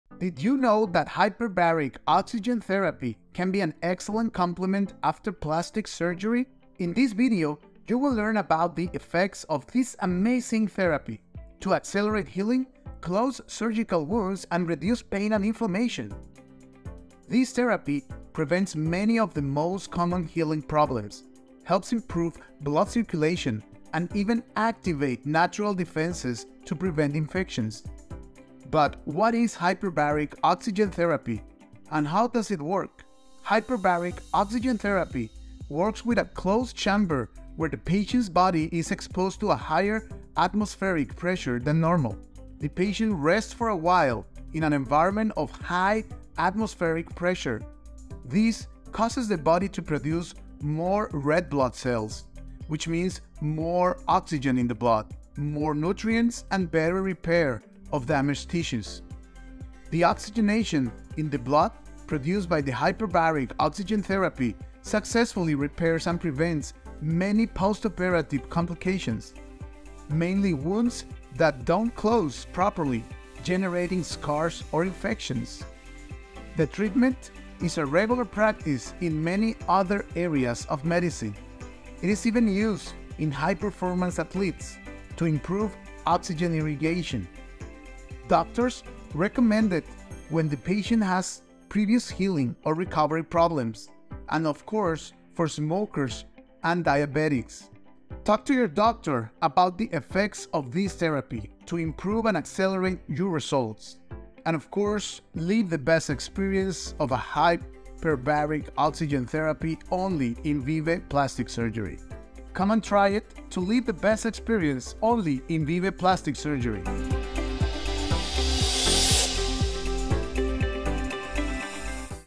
Hyperbaric Chamber Benefits! Learn about the impressive healing technology ✨ - Vive Plastic Surgery
The benefits of the Hyperbaric Chamber extend from pre-operation to post-operation.
You can heal in a better way among other great changes for the better! Thanks to the hyperbaric chamber one can avoid infections, poor healing and more!
Follow us on networks to know all the benefits of the hyperbaric chamber